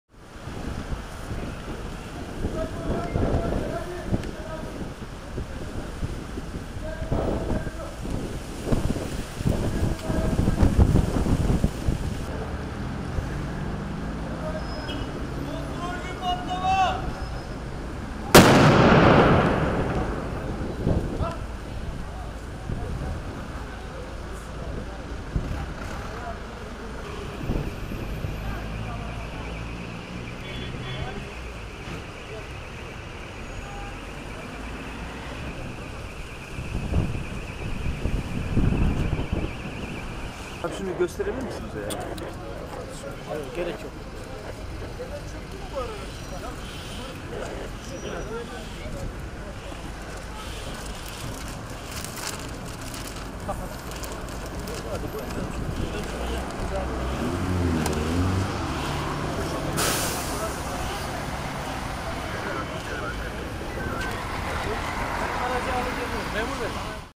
Şüpheli paket fünyeyle patlatıldı, alevler korkuttu
Küçükçekmece'de bir alışveriş merkezinin karşısındaki dershanenin önüne bırakılan şüpheli paket paniğe neden oldu. Bomba uzmanı ekipler şüpheli paketi fünye ile patlattı. Kutuda çakmak gazı olması nedeniyle fünye patladığı an yükselen alevler korkuttu.